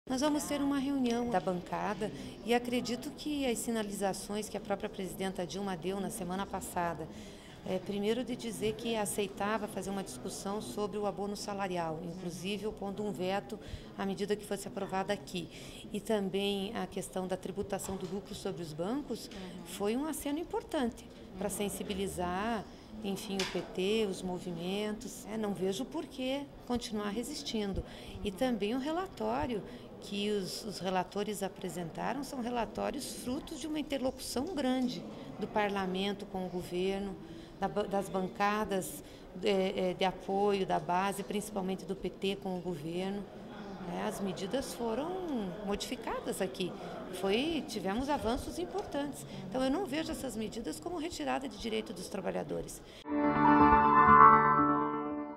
Bancada do PT se reúne nesta terça para discutir votação de MPs do ajuste
Uma reunião de senadores do PT nesta terça-feira (26) buscará a unidade do partido na votação das medidas provisórias do ajuste fiscal.  A senadora Gleisi Hoffmann (PR) disse que ainda espera que parlamentares contrários às MPs mudem de opinião. Na semana passada, os senadores Paulo Paim (RS) e Lindbergh Farias (RJ) anunciaram que votarão contra. As MPs 664 e 665, que mudam as regras de acesso ao seguro desemprego, ao abono salarial e à pensão por morte, perdem validade no dia 1º de junho.